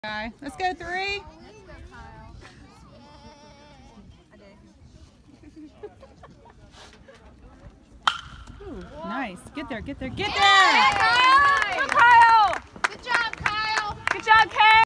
Kyle Horton 2018 OF/RHP - Infield Single
3/18/2017: Kyle Horton,  a 2018 OF/RHP from the Liberty Christian Academy in Lynchburg, Virginia, singles through the 5/6 hole here vs Miller School and gets down the line in 4.12 seconds.